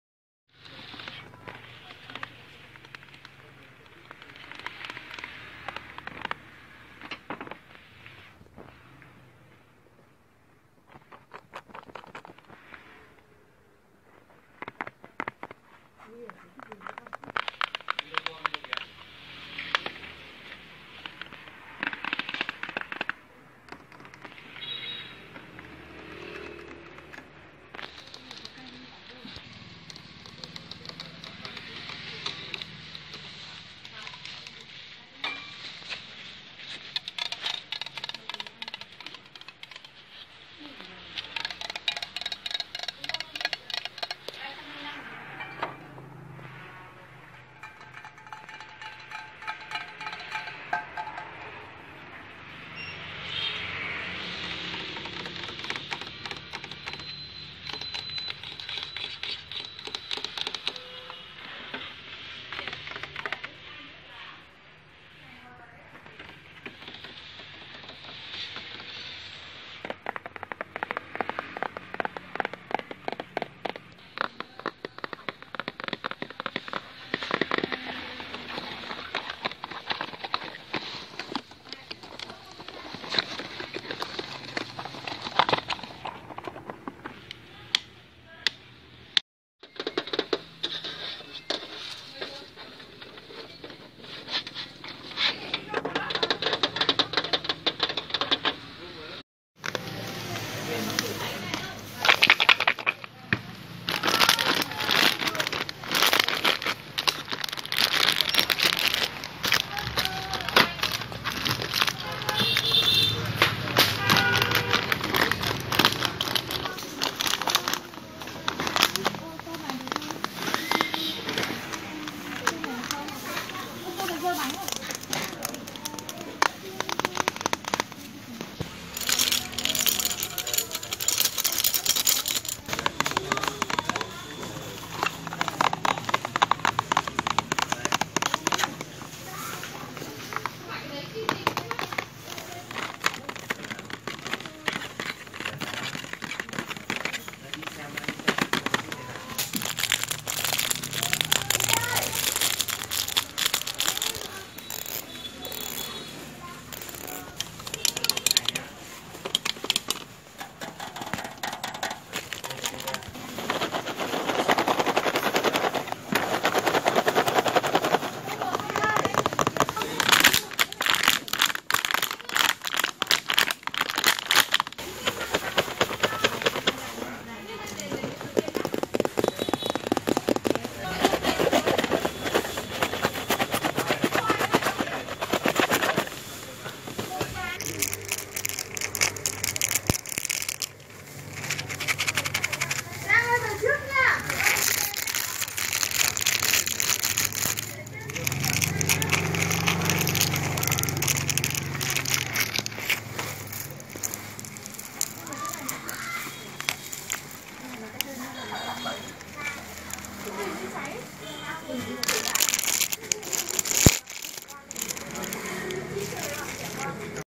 ASMR ~ At the mall
Now in today video I am going to be doing a really fun video . I am tapping and scratching some objects at the mall . I am really sorry about the noise . By the way I hope you guys enjoy this video .